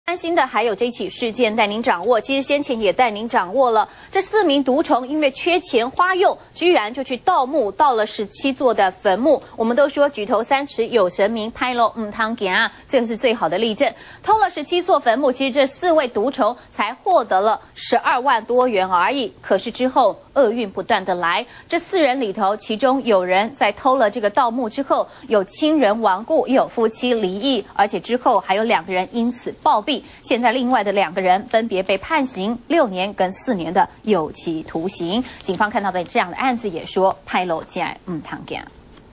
盜墓變衰鬼 4毒蟲2暴斃2判刑
中視新聞》日前警方獲破一個4人盜墓集團，在彰化縣的鄉鎮公墓破壞至少17座墳墓，為竊取死者陪葬的金飾變賣花用。結果這個盜墓集團中的成員死的死、傷的傷，邪事不斷，其中一名嫌犯在應訊時表示自己再也不敢盜墓。